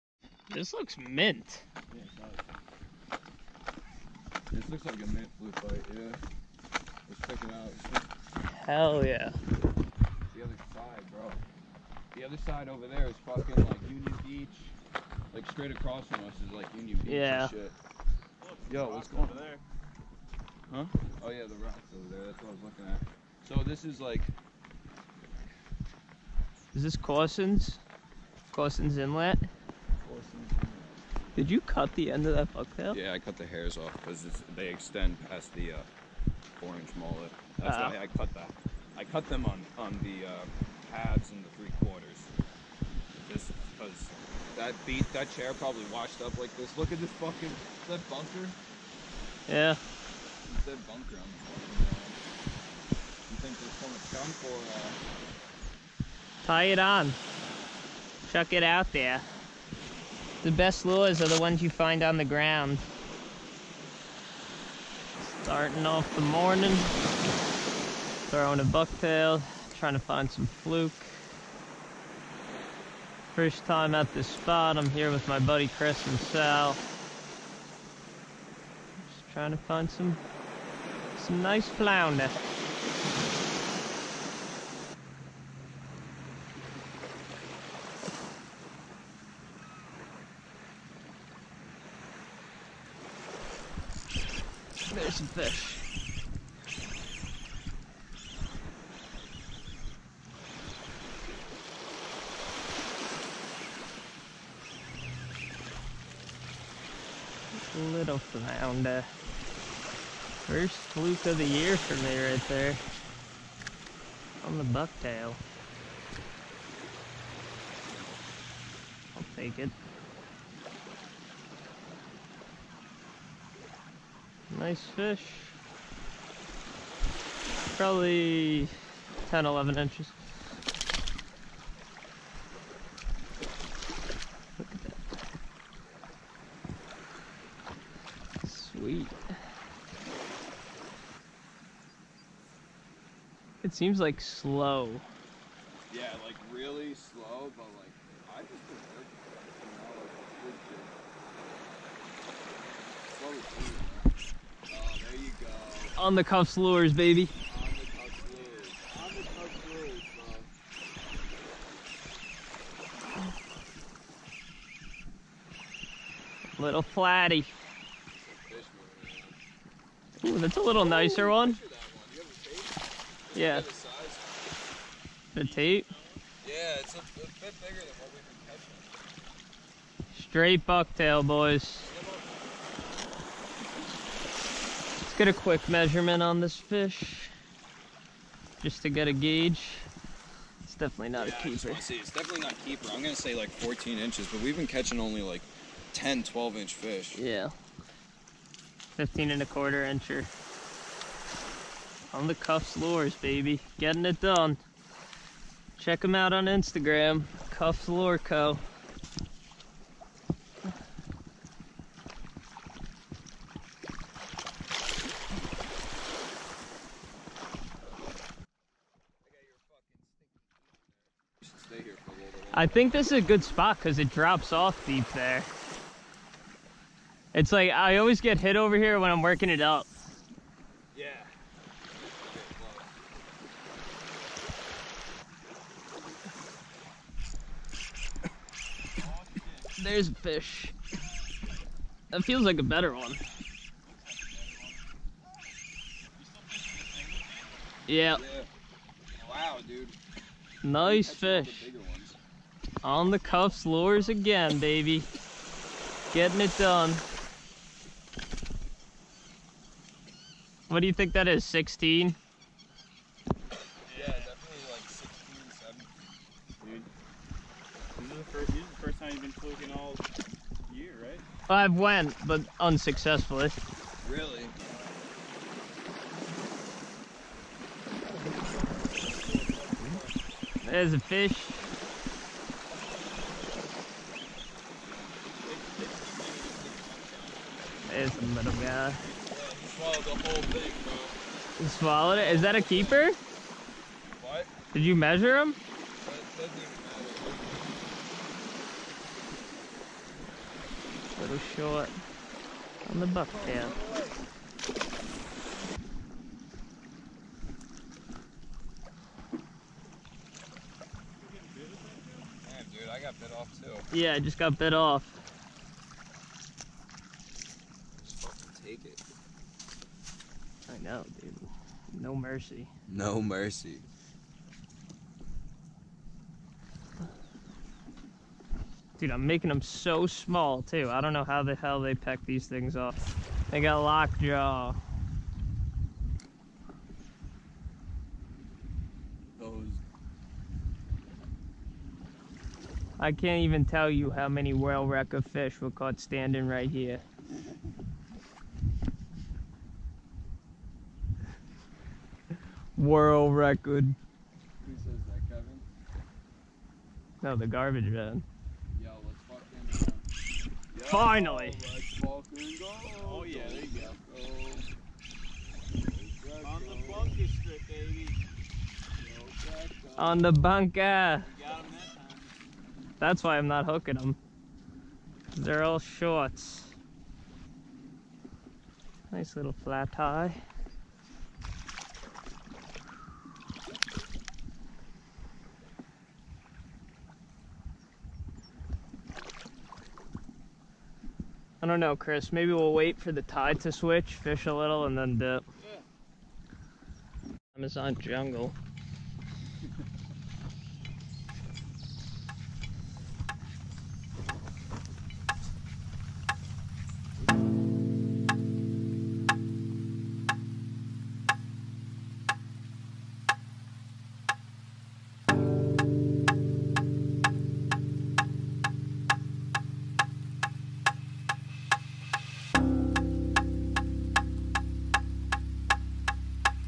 Light Tackle Surf Fishing For Summer Flounder!
Went to explore some new water in search of summer flounder with my buddies Sal and Chris. Got on a nice bite throughout the day and caught some nice fish, none for the table but it was still fun fishing new water!